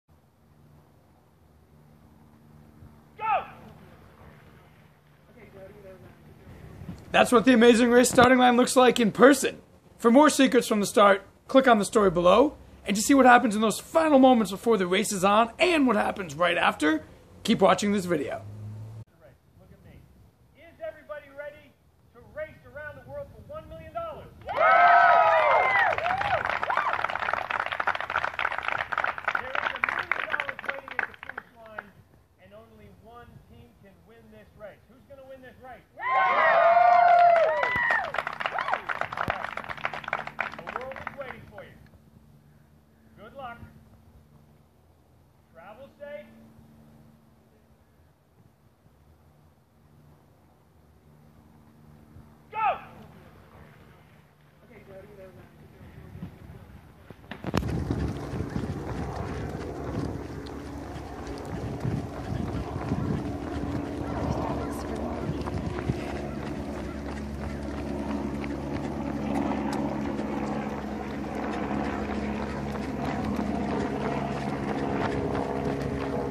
The Amazing Race - Behind the Scenes At the Starting Line "Blind Date"
Place: Castaic Lake, CA
Date: November 12, 2014
Event: The starting line of "The Amazing Race", Season 26

"The Amazing Race's" 26th Season, featuring "Blind Daters" and New Kid on the Block Jonathan Knight started at Lake Castaic north of Los Angeles.

Mikey Glazer got the behind the scenes story and exclusive interviews with the cast and producers before the start.

Follow @TheWrap and @CelebSightings on Twitter